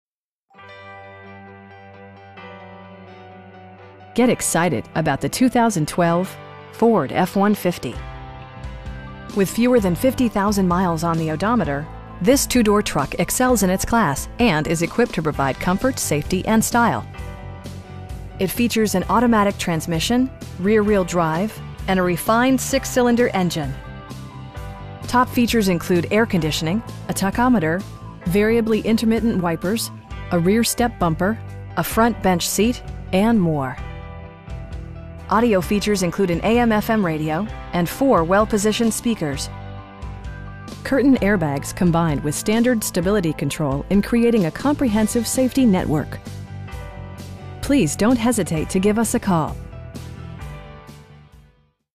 2012 Ford F-150 XL in Sioux Falls, SD 57106
4901 W. 26th St

Get excited about the 2012 Ford F-150. With fewer than 50,000 miles on the odometer, this 2 door truck excels in its class, and is equipped to provide comfort, safety and style. It features an automatic transmission, rear-wheel drive, and a refined 6 cylinder engine. Top features include air conditioning, a tachometer, variably intermittent wipers, a rear step bumper, a front bench seat, tilt steering wheel, and more. Audio features include an AM/FM radio, and 4 speakers, providing excellent sound throughout the cabin. Curtain airbags combine with standard stability control in creating a comprehensive safety network. We pride ourselves in the quality that we offer on all of our vehicles. Please don't hesitate to give us a call.